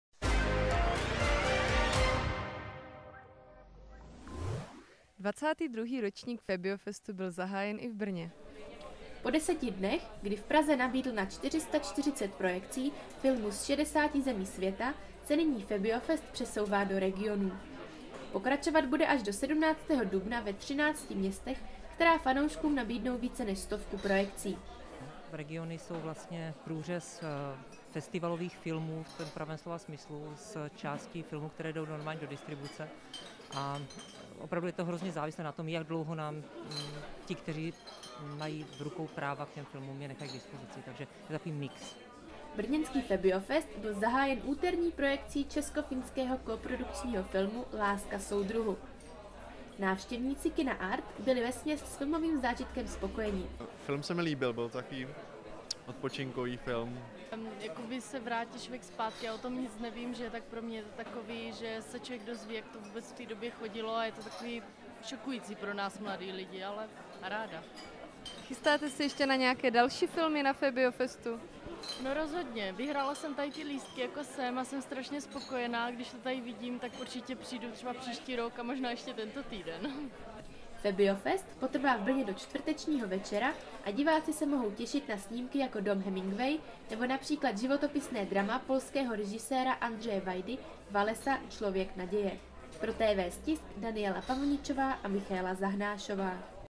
Febiofest dorazil i do Brna
XXI. ročník Febiofestu byl v úterý zahájen i v Brně. Diváci brněnského kina Art mohli zhlédnout snímek Láska, soudruhu. Festival potrvá tři dny a návštěvníkům nabídne celkem děvět filmů Těšit se monou například na černou komedii Dom Hemingway nebo na polské drama Walesa: člověk naděje.